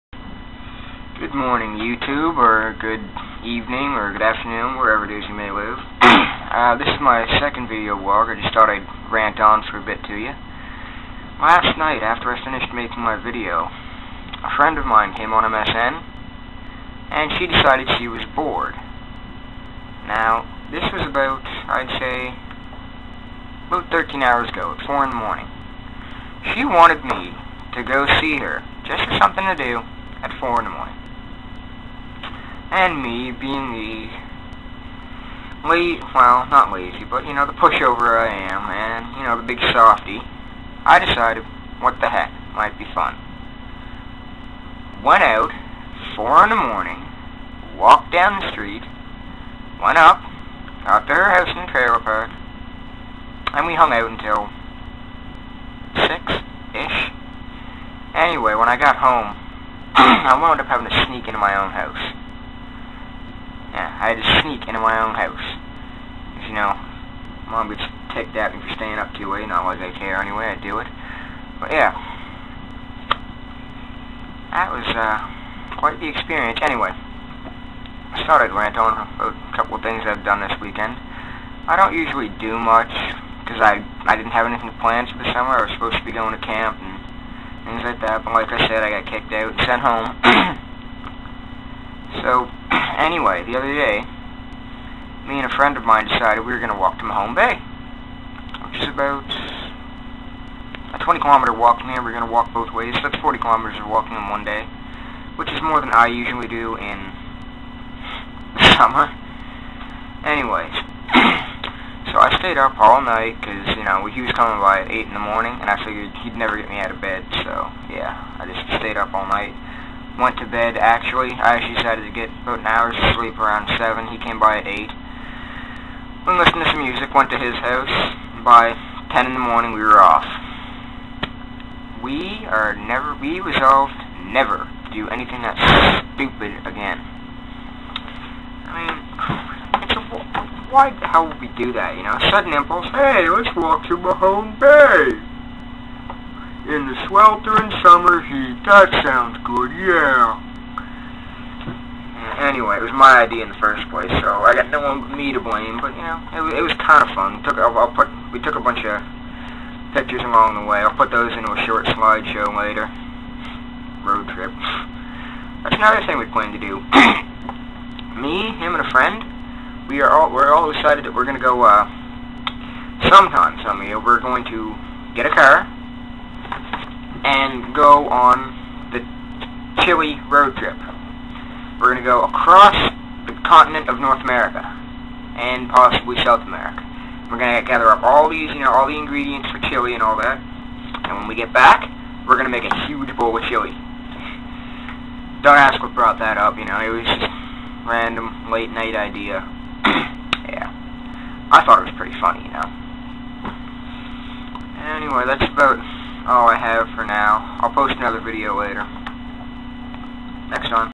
Roadtrip!
Me ranting about my late night experience and road trips :3